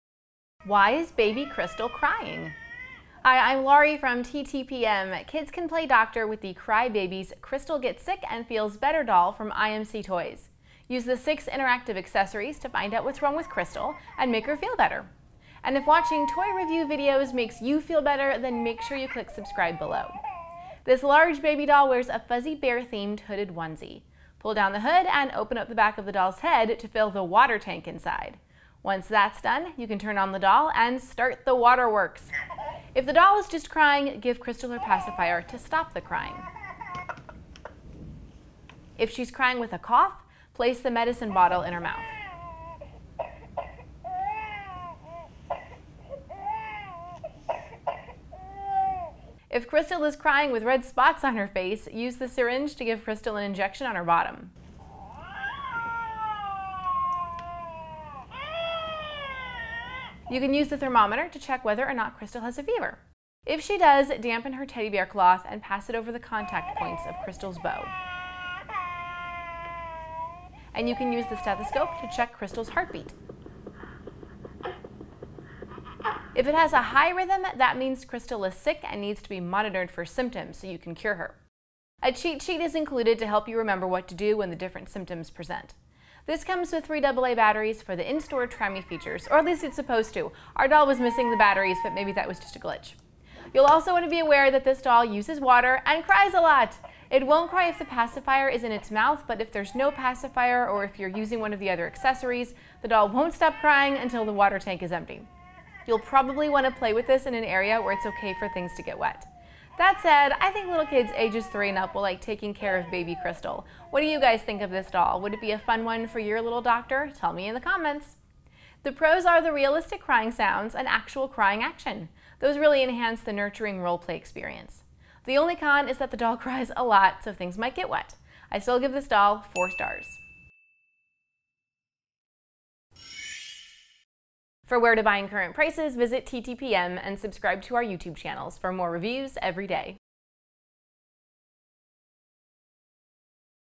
Cry Babies Kristal Gets Sick & Feels Better Doll from IMC Toys
NEW Cry Babies Kristal Gets Sick & Feels Better from IMC Toys. Help baby Kristal feel better with six interactive accessories. This large baby cries tears when you fill her water tank with water. And a cheat sheet helps you detect how to treat the doll's different symptoms: cough, fever, red spots on her face. See more in this video review! For full review and shopping info

Product Info: Why is baby Kristal crying? Kids can play doctor with the Cry Babies Kristal Gets Sick & Feels Better doll. Use the six interactive accessories to find out what's wrong with Kristal and make her feel better.